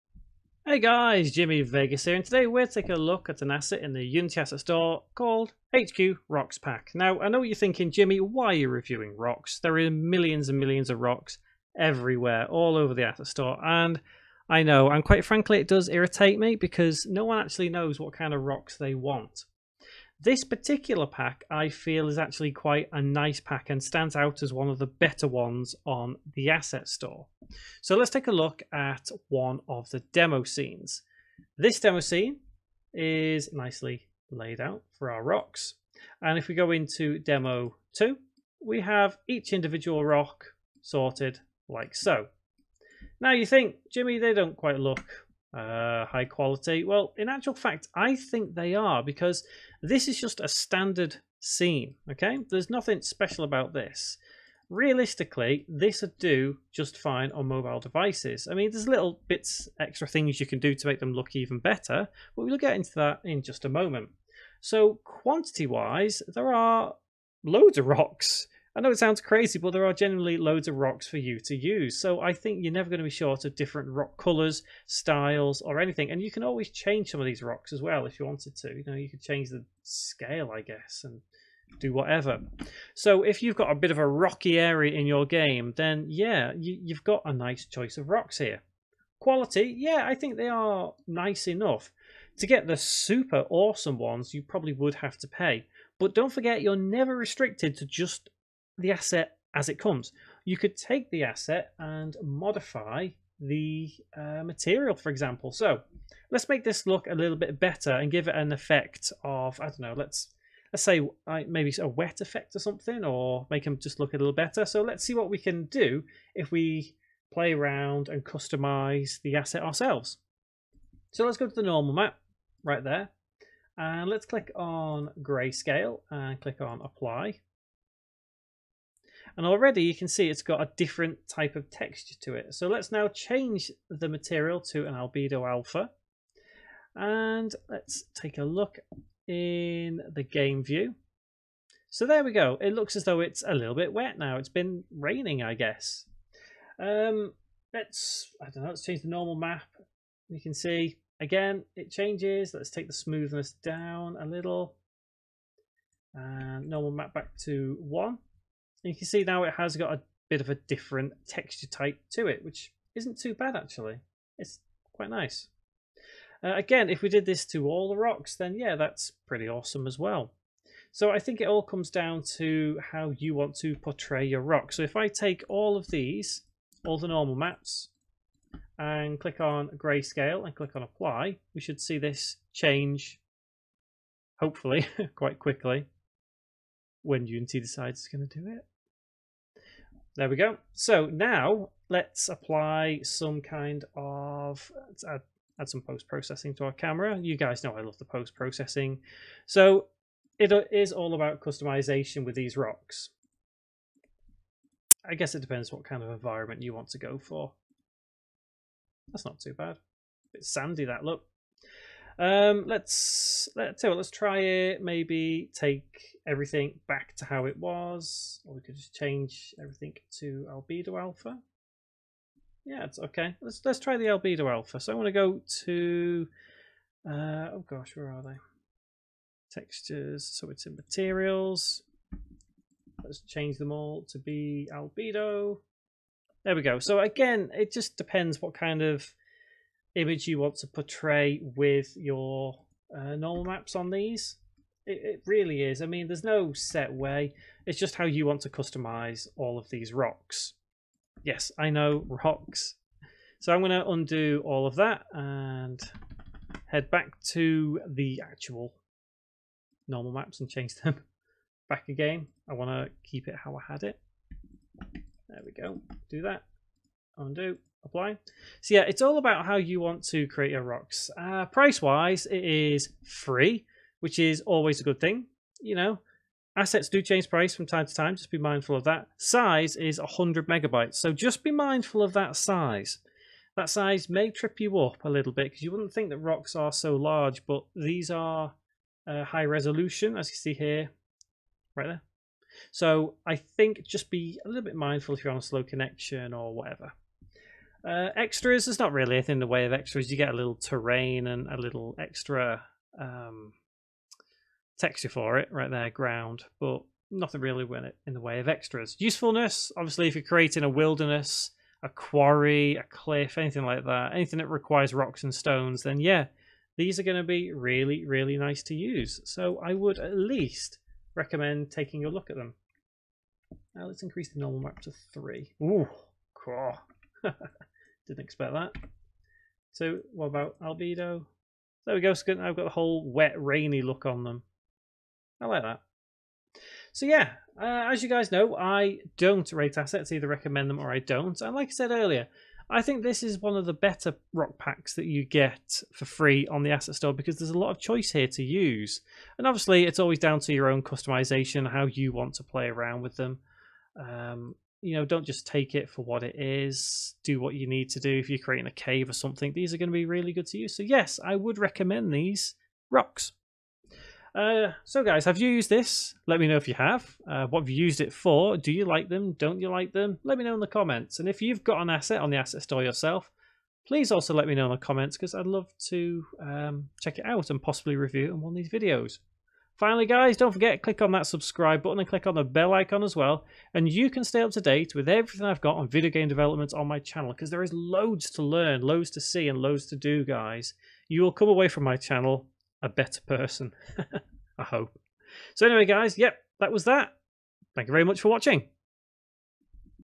UNITY ASSET REVIEW | HQ ROCKS PACK | INDEPENDENT REVIEW BY JIMMY VEGAS ASSET STORE
This pack contains 24 rock prefabs. Review conducted independently and no money was received for this review.

Who Is Jimmy Vegas?